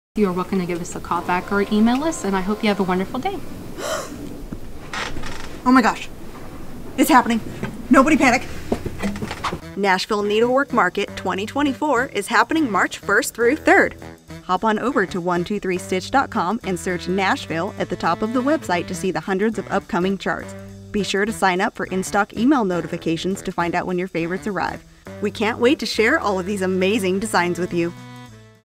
Nashville Needlework Market 2024! It's Happening!!
🧵✨ Exciting News! Nashville Needlework Market 2024 is happening March 1st-3rd! 🎉 Dive into a treasure trove of creativity with hundreds of new patterns from over 70 designers. Explore our website for a sneak peek and sign up for in stock email notifications to be the first to know when these stunning designs hit the shelves in early March. Don't miss out on the stitching event of the year! 🌟